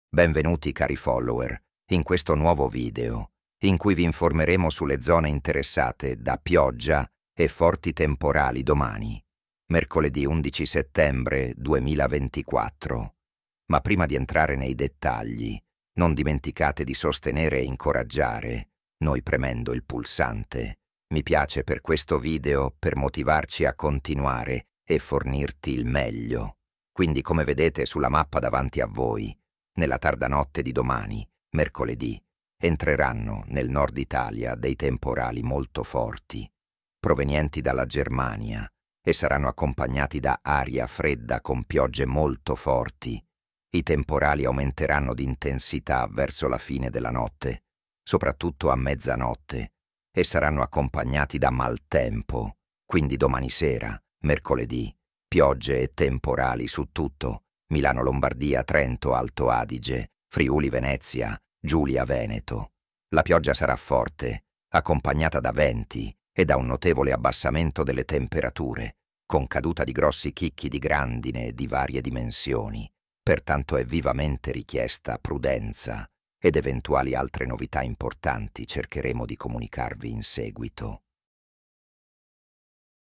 Avviso urgente Zone interessate da forti temporali, pioggia e maltempo domani, mercoledì...
meteo Italia